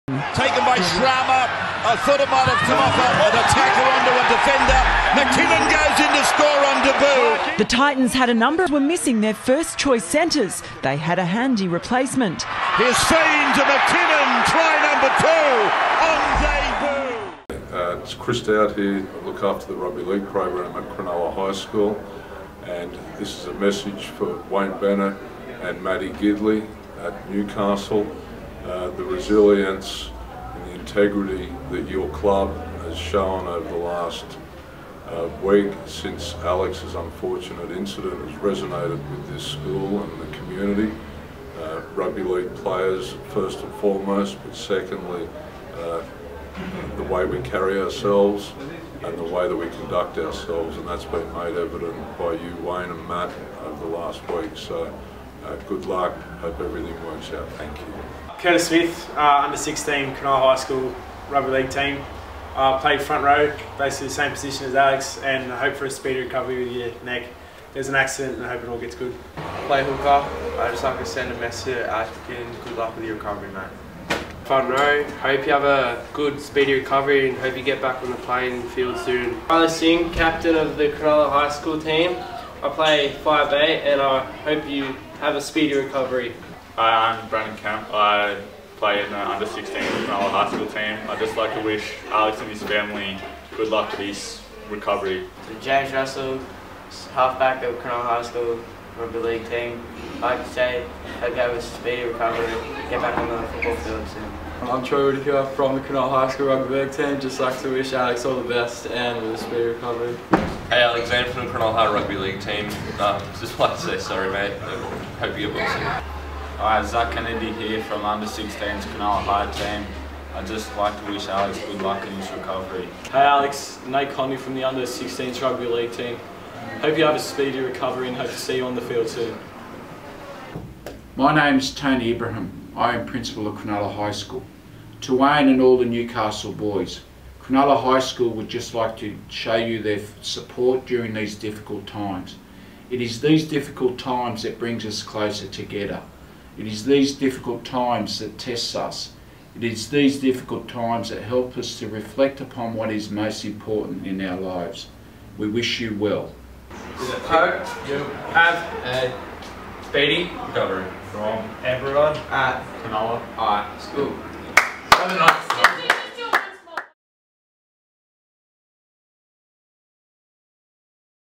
Rise for Alex
A tribute to NRL player Alex McKinnon from Cronulla High School.